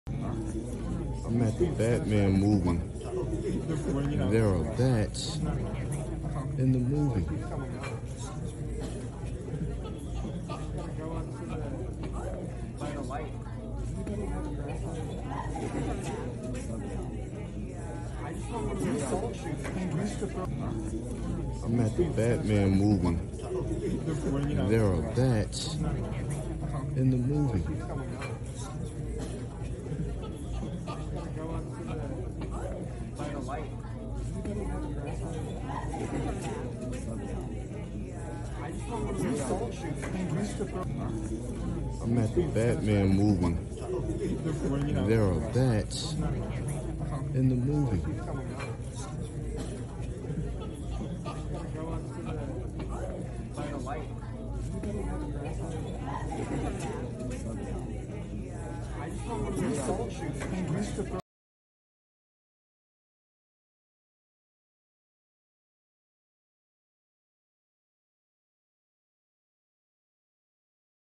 Chaos as live BATS released at The Batman movie screening
Staff was forced to pause the film during a premiere screening on Friday upon realizing that a bat had been released and was flying around the theater. In a short video clip posted on Twitter Saturday, theater staff can be heard telling the audience how they planned on baiting the wild animal in an effort to get the bat out of the building. 'We will use the projector to turn that light off (the movie screen) and I'm gonna turn all these lights off and try and get it to go into this lighted area (an exit),' an employee tells the guests. 'If you are uncomfortable being in the dark with a bat, please remove yourself now,' the theater worker says as the crowd laughs nervously in response.